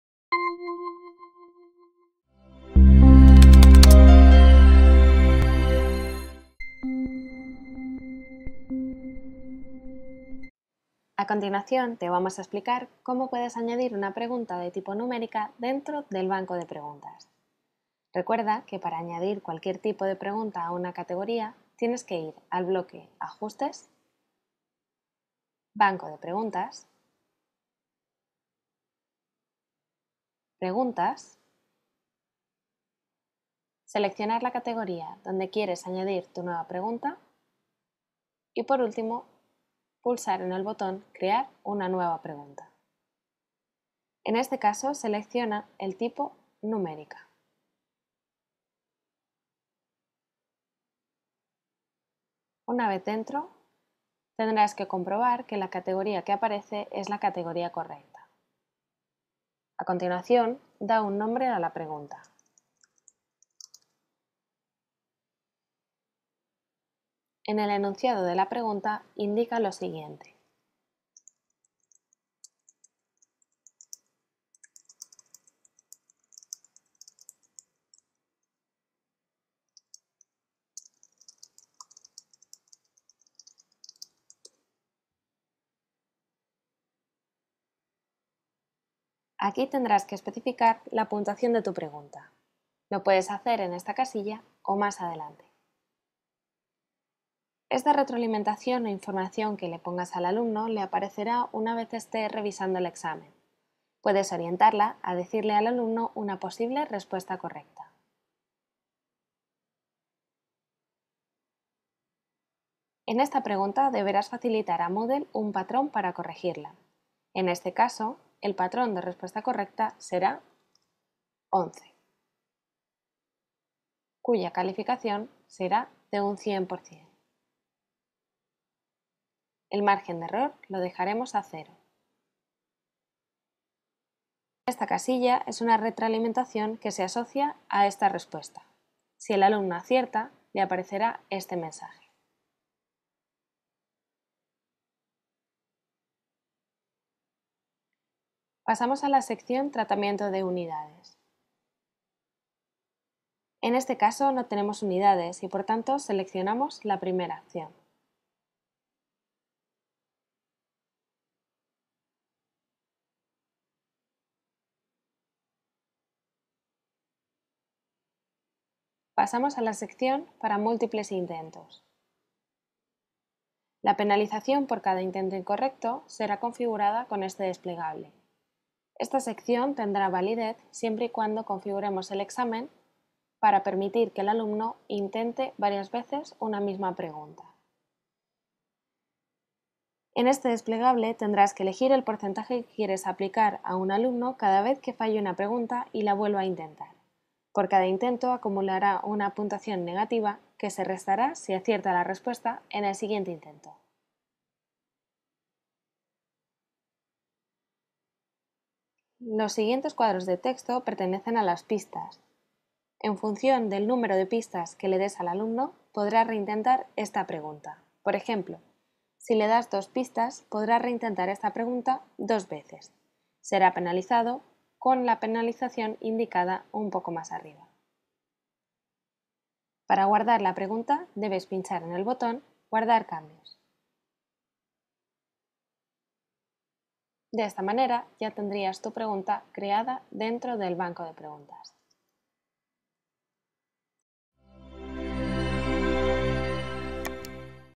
Crear pregunta tipo "Numérica" (Moodle 2.6)
Vídeo explicativo sobre cómo crear el tipo de pregunta "Numérica" dentro del banco de preguntas de una asignatura. En el vídeo se explica cómo crear y configurar este tipo de pregunta dentro de la plataforma Moodle.

Curso Avanzado de Moodle Moodle 2.6 URJC